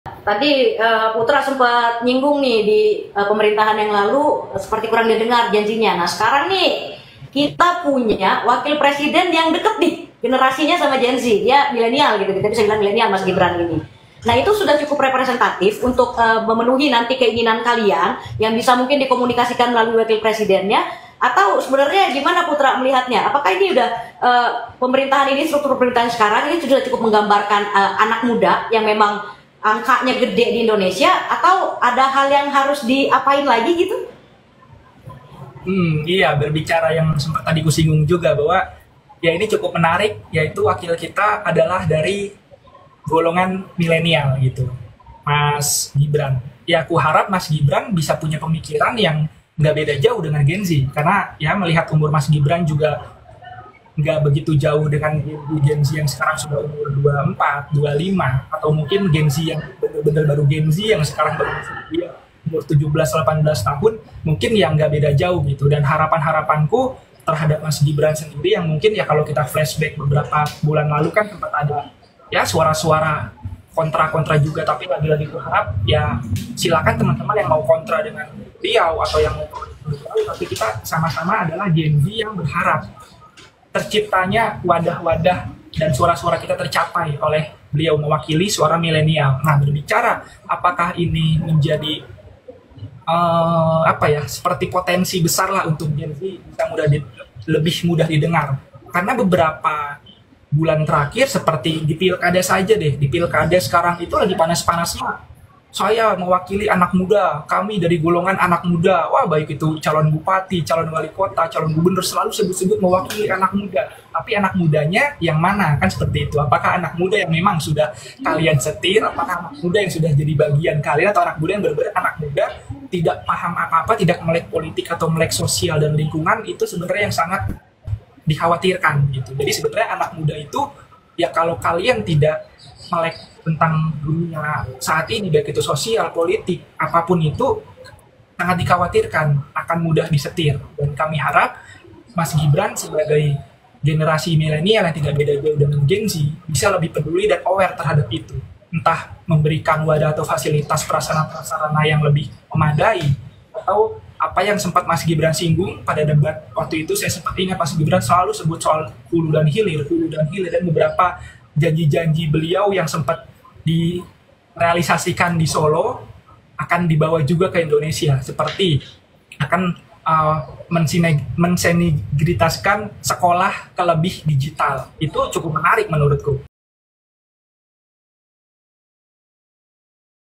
KOMITMEN 100 HARI KERJA PRABOWO GIBRAN DI MATA GEN Z
Pemerintahan Prabowo-Gibran yang telah dilantik, menjadi tantangan besar agar kepemimpinan ini mampu menciptakan kebijakan yang berpihak pada kemajuan generasi penerus bangsa dan mampu merespons dinamika global, serta menangani masalah kesenjangan sosial yang masih terjadi. Generasi muda berharap Pemerintahan Prabowo-Gibran mampu membawa perubahan nyata dan memberi ruang bagi aspirasi mereka untuk terwujud.

"Yang diharapkan mas Gibran sebagai generasi mileniel yang tidak beda jauh dengan gen z, harus bisa lebih peduli seperti janji-janji mas Gibran yang  pernah singgung di debat waktu itu selalu sebut huru dan hilir," ujar Influencer Isu Sosial & Politik/ Ketua DPR Gen Z, Putra Aji Sujati.